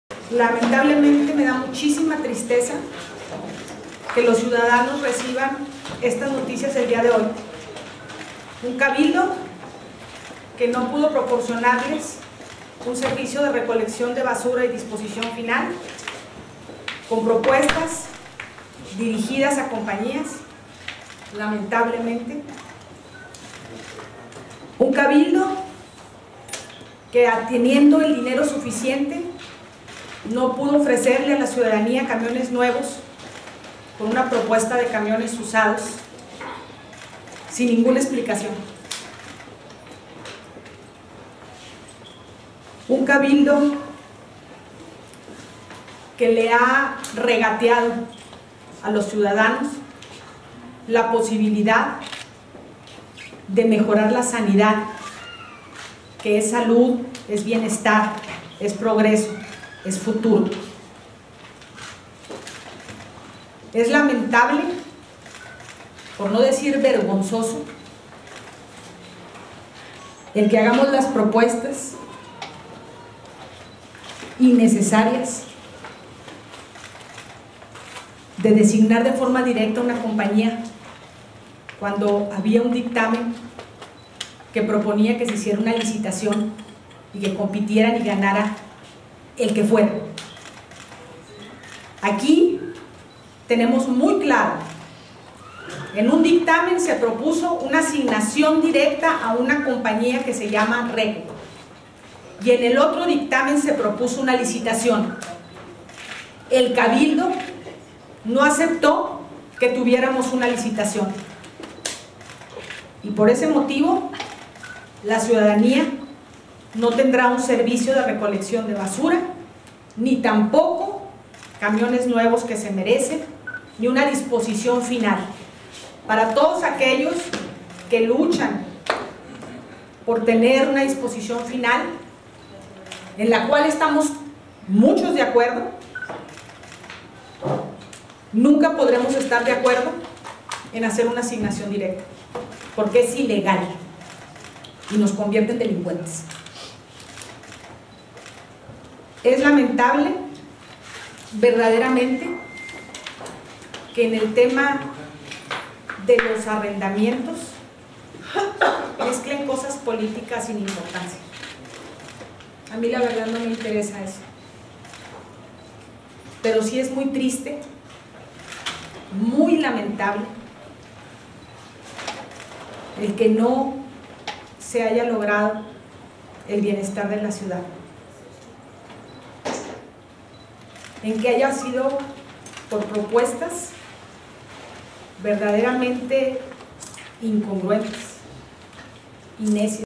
Negativa a Programa de adquisiciones y de arrendamiento para el ejercicio 2017
En maratónica sesión de Cabildo realizada este martes 28 de Marzo en Reynosa y ante la falta de acuerdos que derivaron en la negativa de aprobación para adquirir 20 camiones nuevos destinados al servicio de recolección de basura, arrendamiento de maquinaria y renta de 11 inmuebles que ubican actualmente oficinas municipales entre ellas se encuentra la Secretaria de Relaciones Exteriores (SRE). La Alcaldesa, Maki Ortiz precisó que analizarán los medios jurídicos para que, tanto el programa de adquisiciones como el de arrendamiento para el ejercicio 2017 sean aprobados a beneficio de la población y no de manera "ilegal, inmoral y falta de ética", como proponen los Regidores y Primer Síndico, Alfredo Castro Olguín.